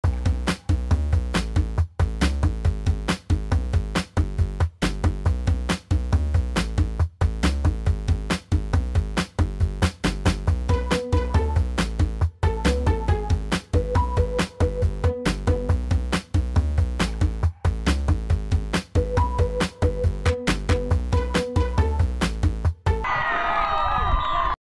Trabuco Hills Girls Lacrosse SCORES!
The Mustang JV first hits the post, then recoups to score a goal at Sticks & Stones Invitational!